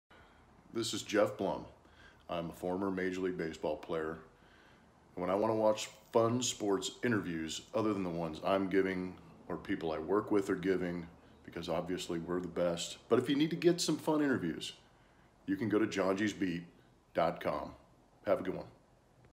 Geoff Blum Shout Out to Johngy's Beat
Former Major League Baseball player Geoff Blum was kind enough to do a fun promo for Johngy's Beat. A long-time favorite of mine, I had the pleasure of interviewing Geoff years ago in Houston at
For more pop culture nuggets, including interviews, pictures, stories and other fun, go